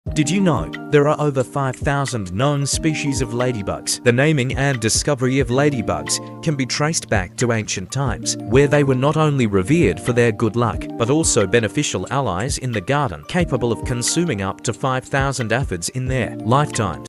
Lady bugs are Good Luck!
Ladybugs, scientifically known as Coccinellidae, are a type of beetle belonging to the family Coccinellidae. These small insects are renowned for their brightly colored bodies, typically red or orange with black spots. Ladybugs are found across the globe, inhabiting various habitats such as gardens, forests, meadows, and even urban areas.

The presence of ladybugs has been appreciated for centuries due to their perceived benefits. Farmers and gardeners welcomed ladybugs as beneficial insects since they feed on plant pests, especially aphids. This natural pest control ability made them valuable allies in protecting crops and plants.

Ladybugs continue to captivate our curiosity with their striking appearance and ecological importance. Their presence in gardens and their role in controlling pests make them beloved insects among nature enthusiasts and gardeners alike.